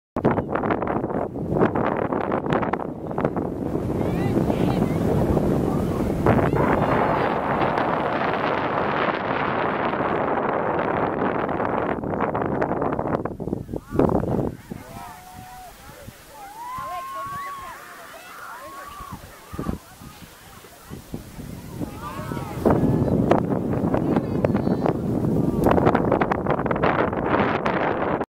White Sands National Monument short clip
A very short clip from White Sands Natl. Monument in NM with an awesome storm cloud.